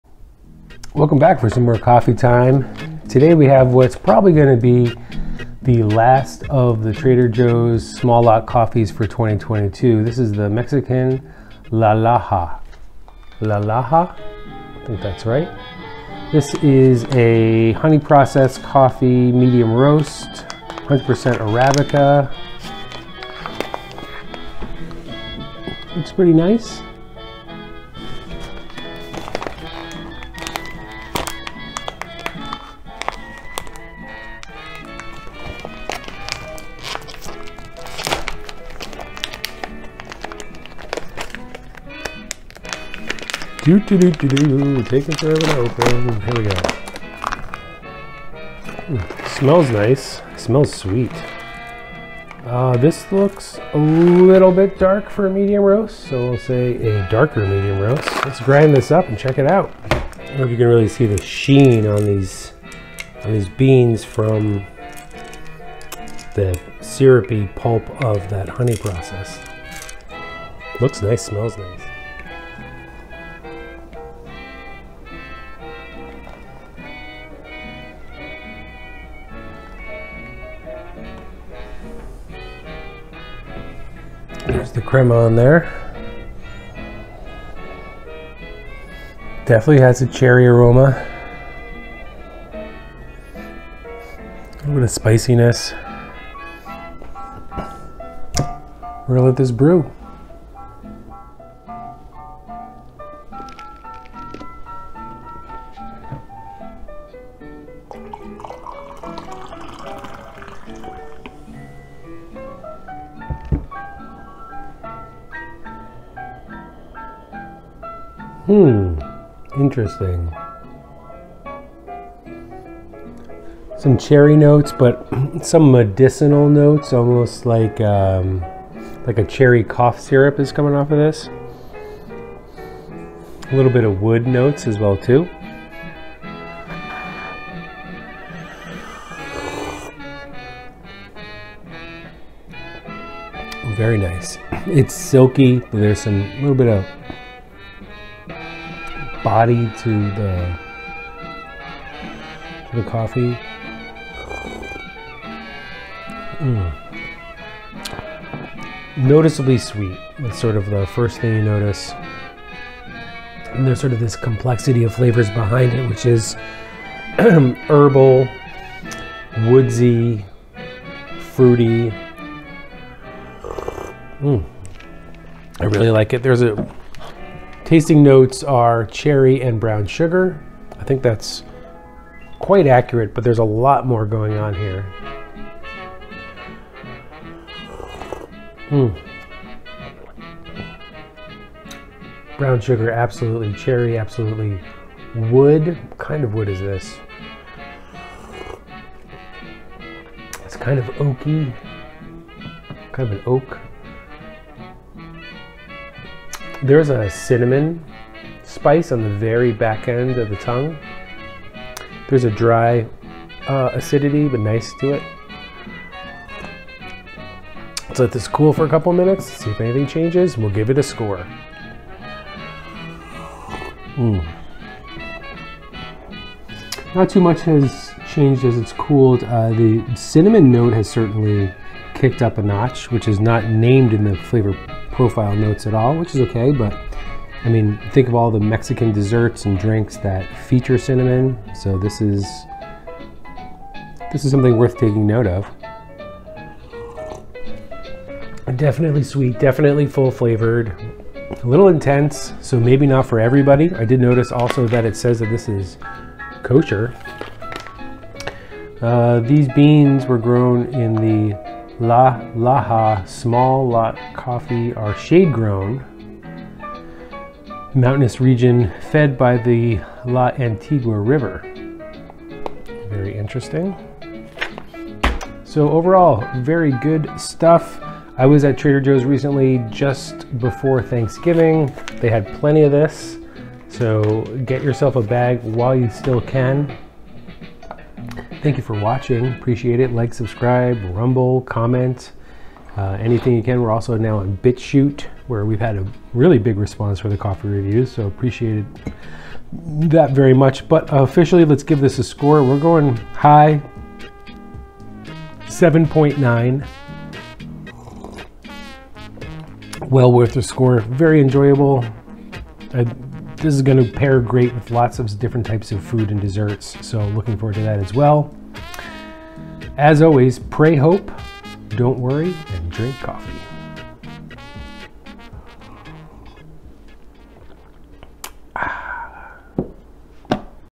Trader Joe's Mexico “La Laja” Small Lot Coffee
The last of the TJ small lot coffee's for 2022. This is a great offering that has been honey processed.
Use my codes for discounts on the Worlds Best Coffee:

Atlas Coffee Club (A great monthly subscription)

Guadalupe Roastery - Coffee For the Common Good